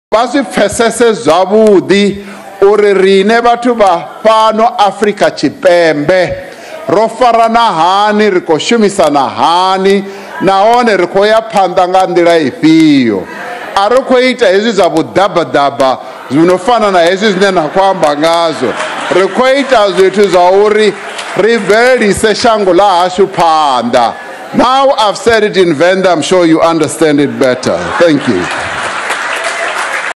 President Cyril Ramaphosa using Venda in parliament
SA President Cyril Ramaphosa